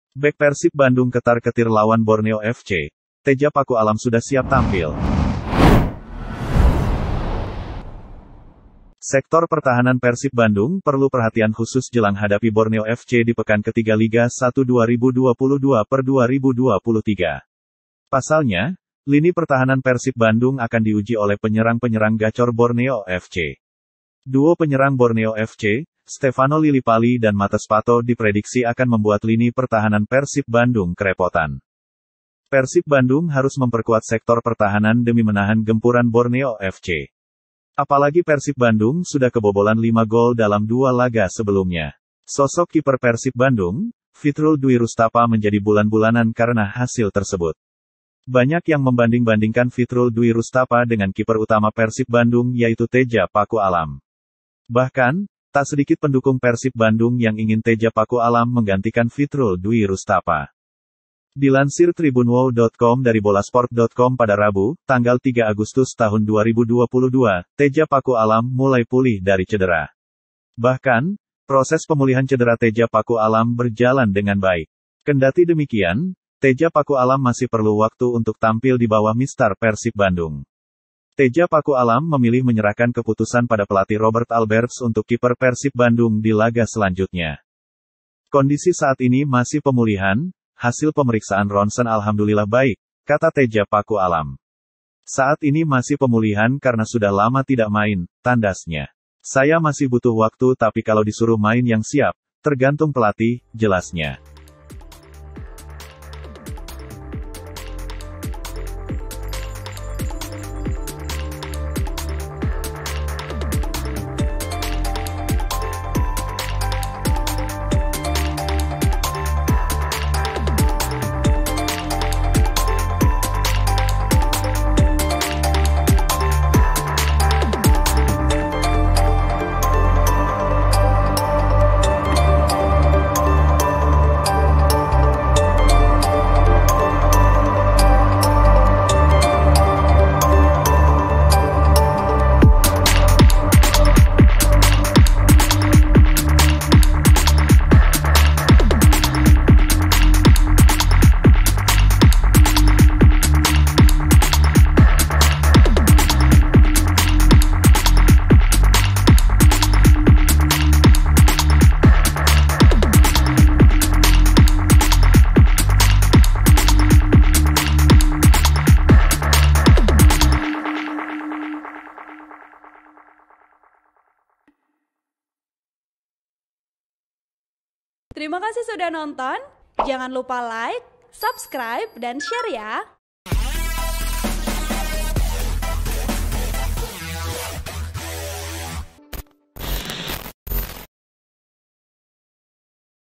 Bek Persib Bandung Ketar-ketir Lawan Borneo FC, Teja Paku Alam Sudah Siap Tampil?
Sektor pertahanan Persib Bandung perlu perhatian khusus jelang hadapi Borneo FC di pekan ketiga Liga 1 2022/2023.

Pasalnya, lini pertahanan Persib Bandung akan diuji oleh penyerang-penyerang gacor Borneo FC.

Duo penyerang Borneo FC, Stefano Lilipaly dan Matheus Pato diprediksi akan membuat lini pertahanan Persib Bandung kerepotan.

Persib Bandung harus memperkuat sektor pertahanan demi menahan gempuran Borneo FC.

Apalagi Persib Bandung sudah kebobolan lima gol dalam dua laga sebelumnya.

Sosok kiper Persib Bandung, Fitrul Dwi Rustapa menjadi bulan-bulanan karena hasil tersebut.

Banyak yang membanding-bandingkan Fitrul Dwi Rustapa dengan kiper utama Persib Bandung yaitu Teja Paku Alam.

Bahkan, tak sedikit pendukung Persib Bandung yang ingin Teja Paku Alam menggantikan Fitrul Dwi Rustapa.

Sayangnya Teja Paku Alam harus menepi memperkuat Persib Bandung karena cedera patah tulang bagian tangan kirinya.

Oleh karena itu, Teja Paku Alam absen dua pertandingan dan gawang Persib Bandung dipercayakan pada Fitrul Dwi Rustapa.

Angin segar menghampiri Persib Bandung terkait kondisi Teja Paku Alam.

Dilansir TribunWow.com dari Bolasport.com pada Rabu (3/8/2022), Teja Paku Alam mulai pulih dari cedera.

Bahkan, proses pemulihan cedera Teja Paku Alam berjalan dengan baik.

Kendati demikian, Teja Paku Alam masih perlu waktu untuk tampil di bawah mistar Persib Bandung.

Teja Paku Alam memilih menyerahkan keputusan pada pelatih Robert Alberts untuk kiper Persib Bandung di laga selanjutnya.

"Kondisi saat ini masih pemulihan, hasil pemeriksaan rontgen alhamdulillah baik," kata Teja Paku Alam.

"Saat ini masih pemulihan karna sudah lama tidak main," tandasnya.

"Saya masih butuh waktu tapi kalau disuruh main yang siap, tergantung pelatih," jelasnya.

(NSA)